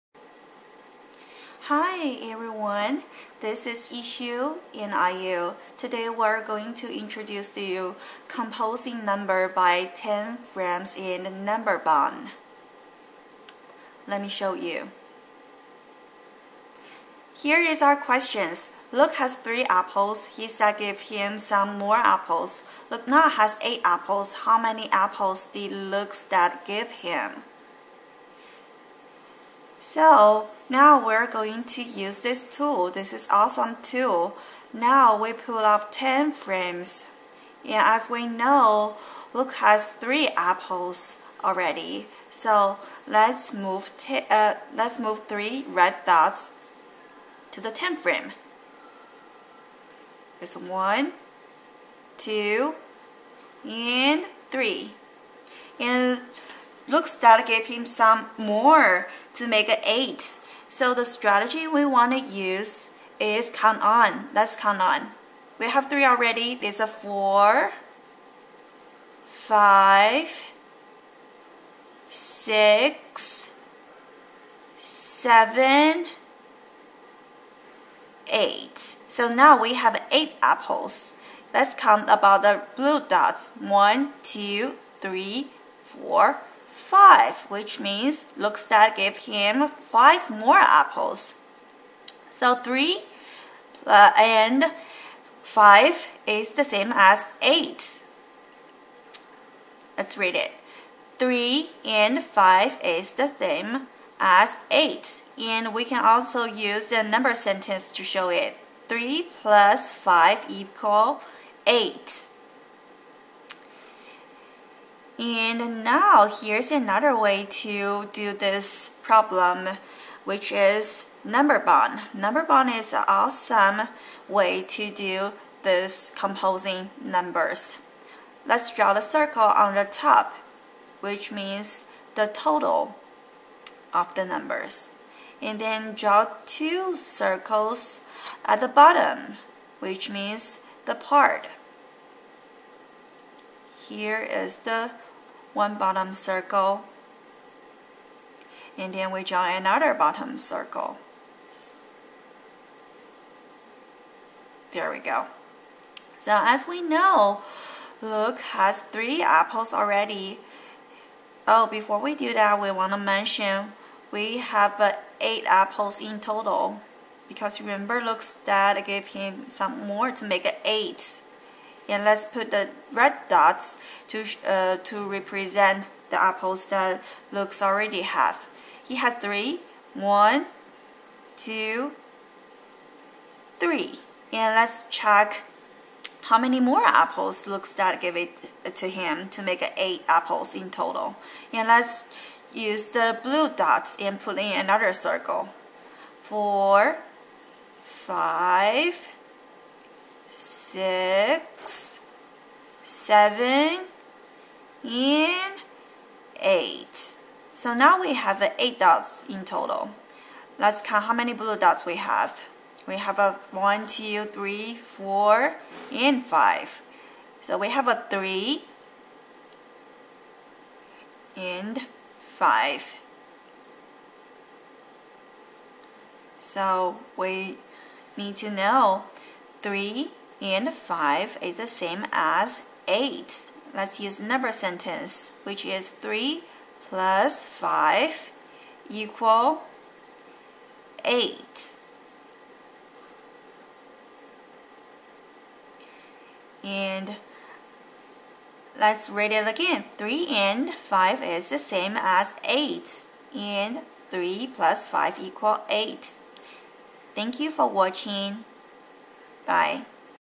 Addition - Join Change Unknown (Ten Frame): Grade 1
TEKS:1.5(D)  represent word problems involving addition and subtraction of whole numbers up  to 20 using concrete and pictorial models and number sentences & 1.5(F)  determine the unknown whole number in an addition or subtraction equation when the unknown may be any one of the three or four terms in the equation

CCSS.MATH.CONTENT.1.OA.A.1
Use addition and subtraction within 20 to solve word problems involving situations of adding to, taking from, putting together, taking apart, and comparing, with unknowns in all positions, e.g., by using objects, drawings, and equations with a symbol for the unknown number to represent the problem.

Created by Yiuxiu Y. & Ayo S.